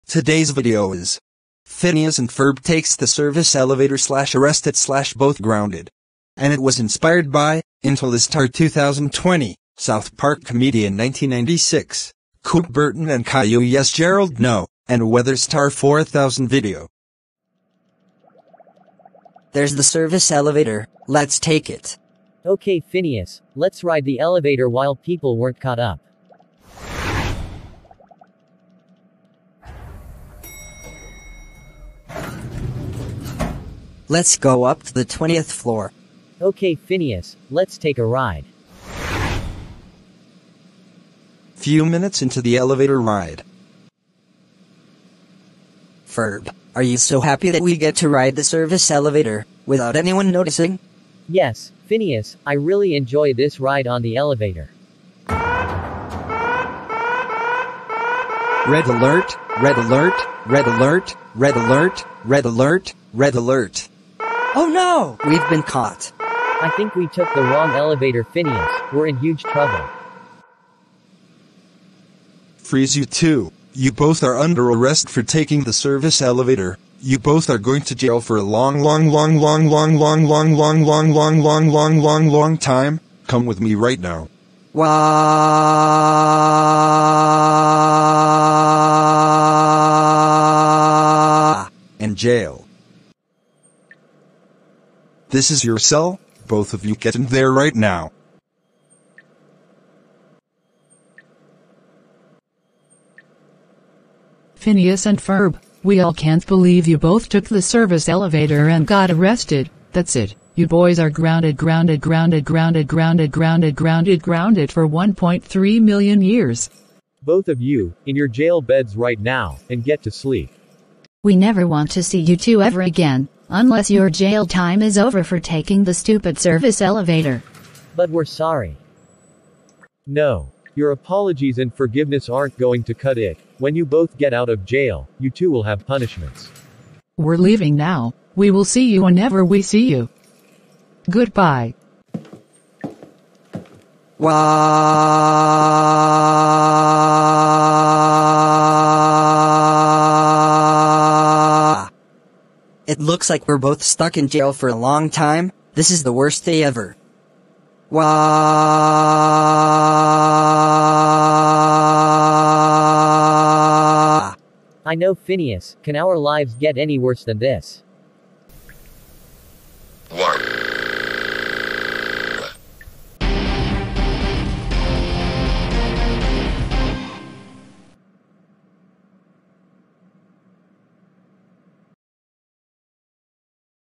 Phineas & Ferb Takes The Service Elevator / Arrested / Both Grounded
Inspired by: @Intellistar2233 @youngman1280 @ethantheultimatekingdomhea1455 and @Weatherstar4000video

Phineas & Ferb were both visiting the Amazon warehouse center, to pick up some packages, But later, they both sneaked into the employment center, to take the Service Elevator. What will happen? Find out...